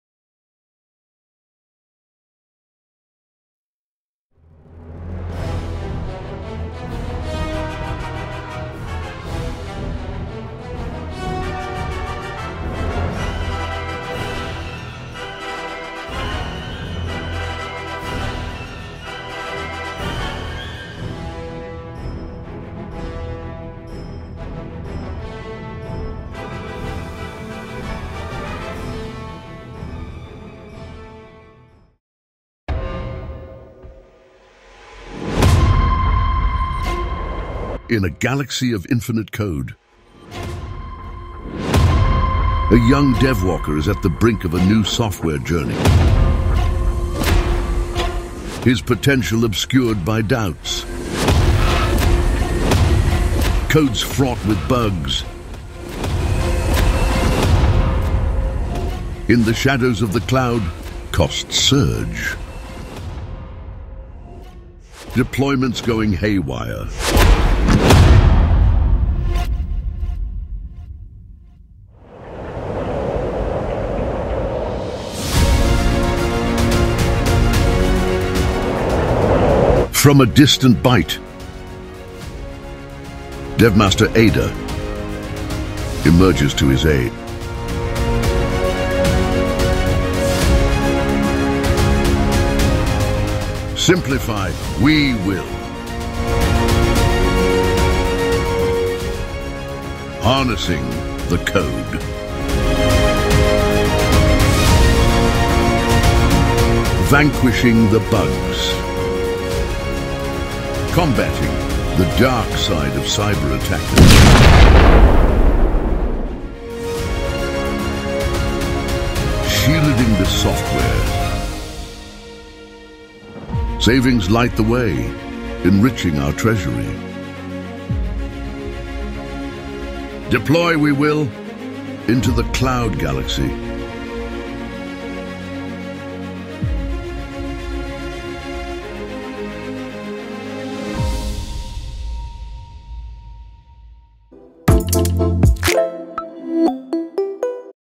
Harness AIDA™ at Unscripted 2023
Watch how Harness AIDA helps DevWalker's experience with AI-infused software delivery.